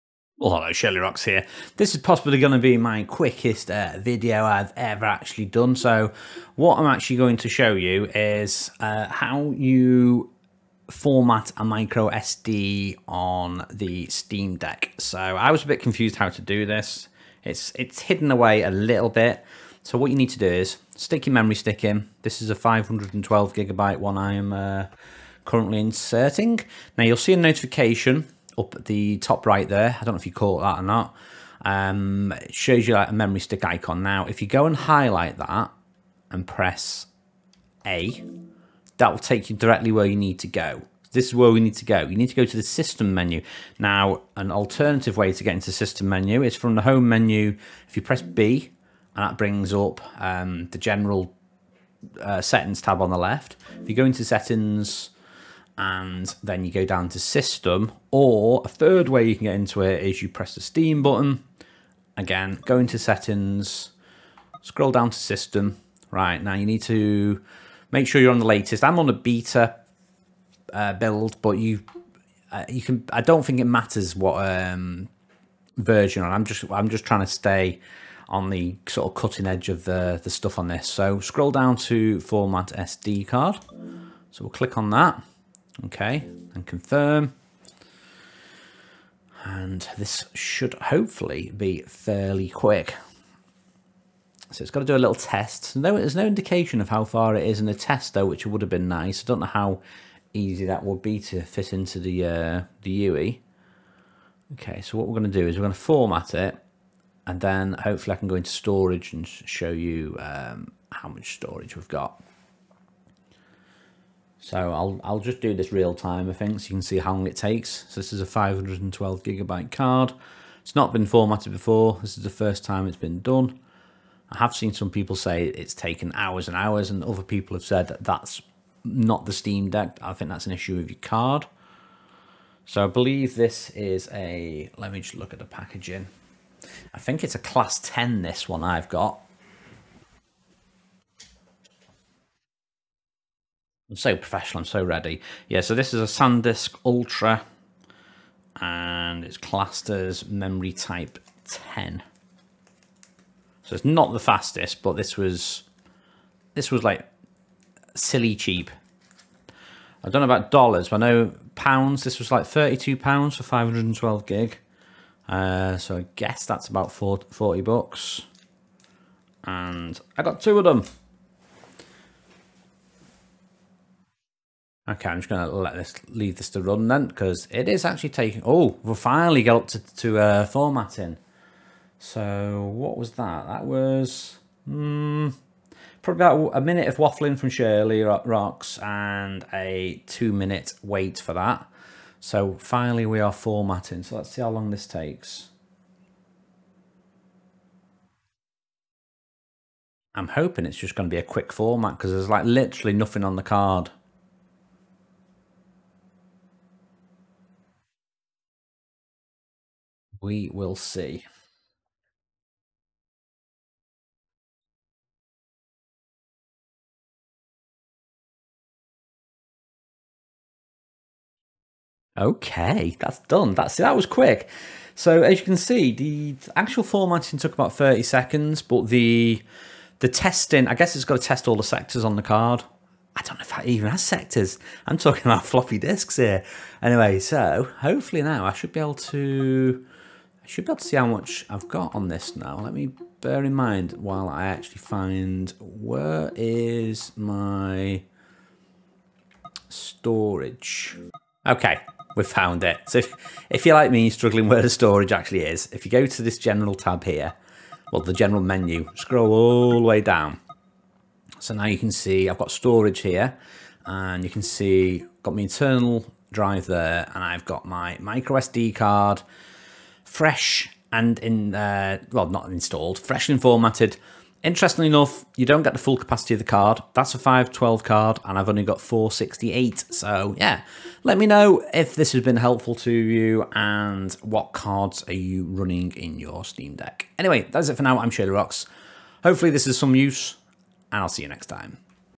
Steam deck micro sd format - how to format micro sd card steam deck
So I recently received a steam deck and was struggling  how to format micro sd card steam deck.

So here is my steam deck micro sd format video that will show you how to format that elusive card within the steam deck